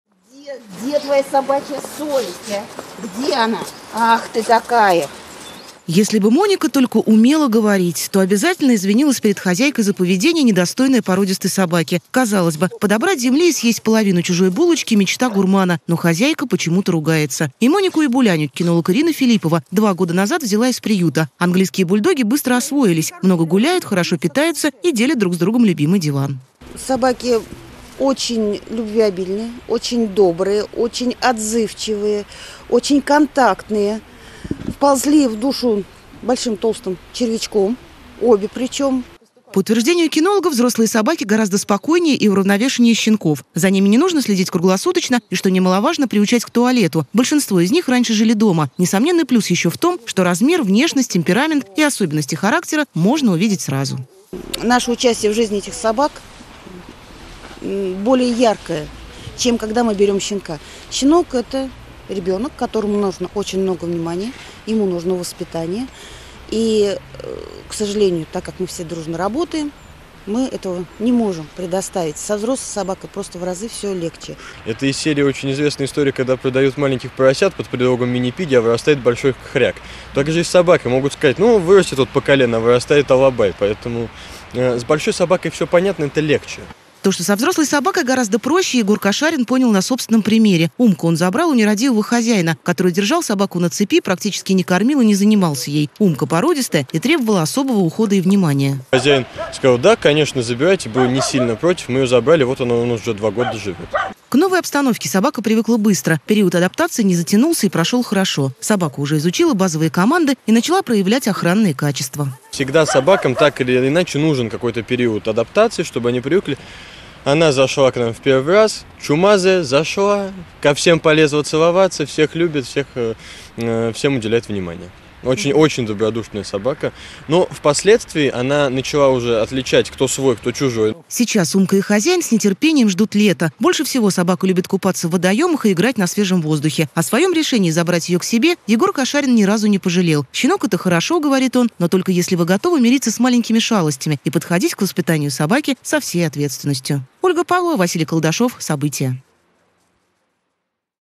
В Самаре стали все чаще брать из приютов не щенков и котят, а вполне сформировавшихся животных
Взрослый — не значит чужой. В Самаре стали все чаще брать из приютов не щенков и котят, которых можно воспитать по-своему, а вполне сформировавшихся животных. По словам кинологов,  плюс такого питомца в том, что его не нужно приучать к лотку или прогулкам. Но при этом нужно считаться с его привычками, сложившимися годами. Животным какого возраста стоит отдать предпочтение, чтобы хвостатый стал по-настоящему родным, выяснила Ольга Павлова.